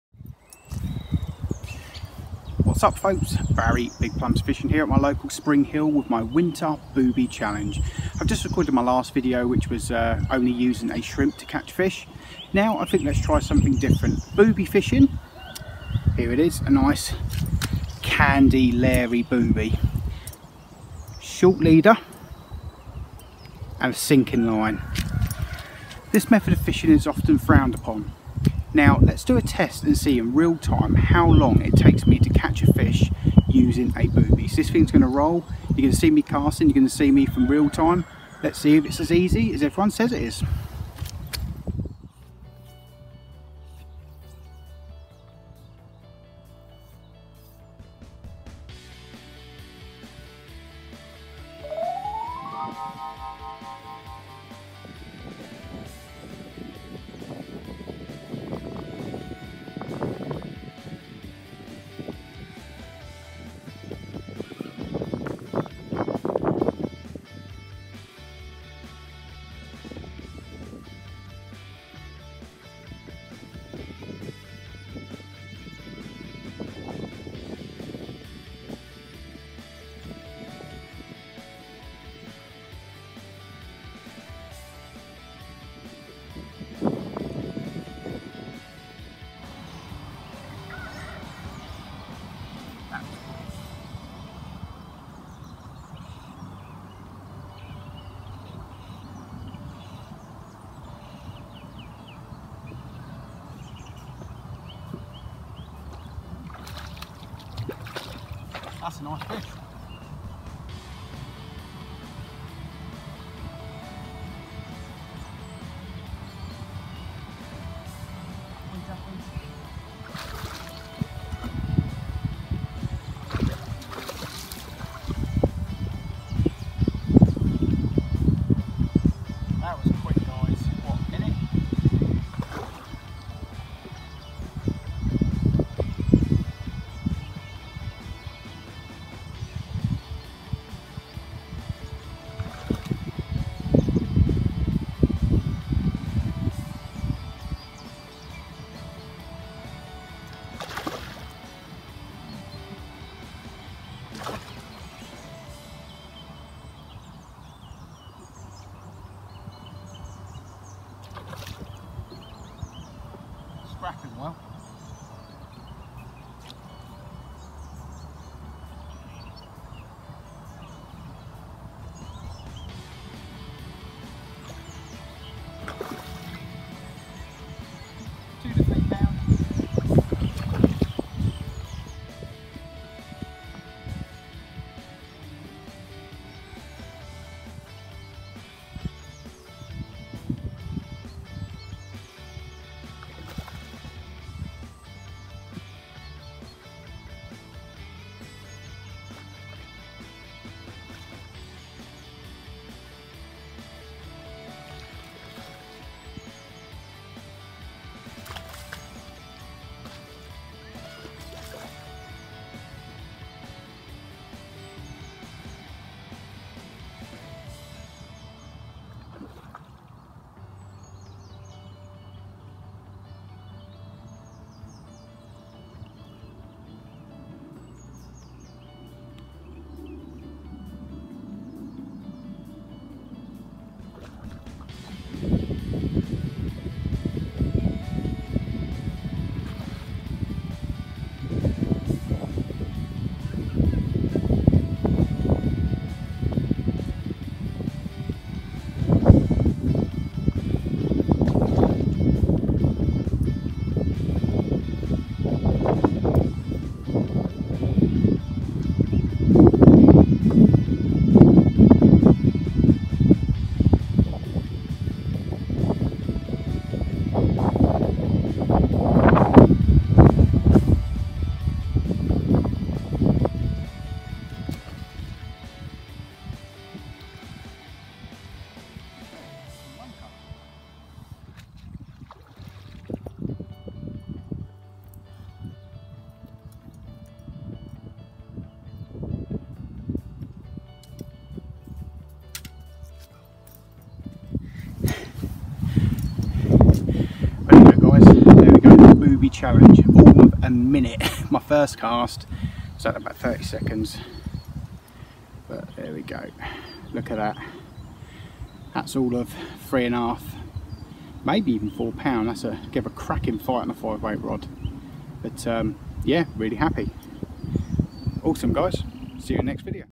How Long To Catch A Trout Using Booby? Barrybigplums Winter Booby Challenge
A session at my local fishery in tumbridge wells, Spring Hill trout fishery.

Here is my winter booby challenge - How quickly catch a trout on a booby?

Its freezing cold it's winter and the fish are down deep in the depths,

One tactic you can use is a booby lure on a deep sinking line, it's very effective and here is a video of giving it a shot on a cold winters day in January!

Xbox Gamertag - BarryBigPlums
PSN ID - k_sakuraba1982

My Buddies

Gamergeekworld/somersetbeastman

Chrismadagames